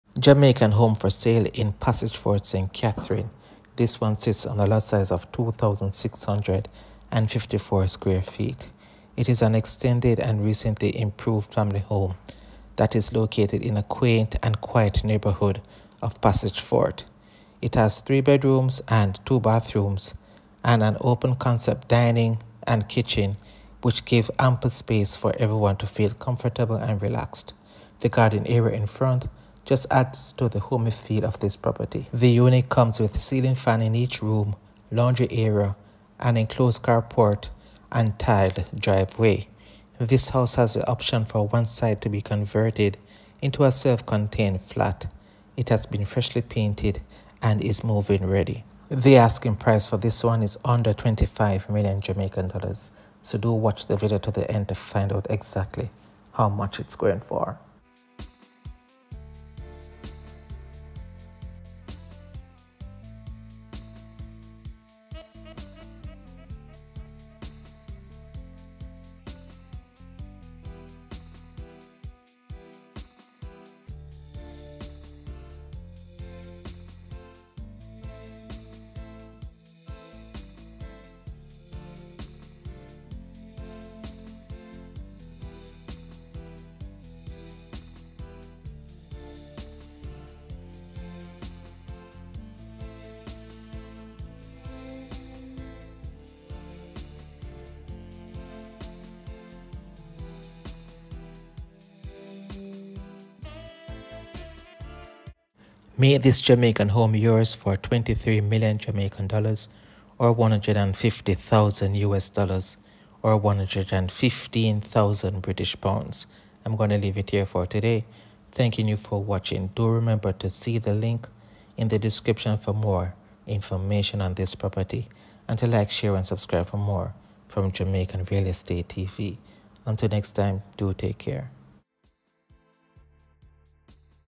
FOR SALE UNDER $25M ST. CATHERINE🇯🇲
House for sale in Passage Fort St. Catherine Jamaica. This property sits on over 2,000 square feet and has 3 bedrooms and 2 bathrooms. The asking price for this real estate is less than $25 million dollars.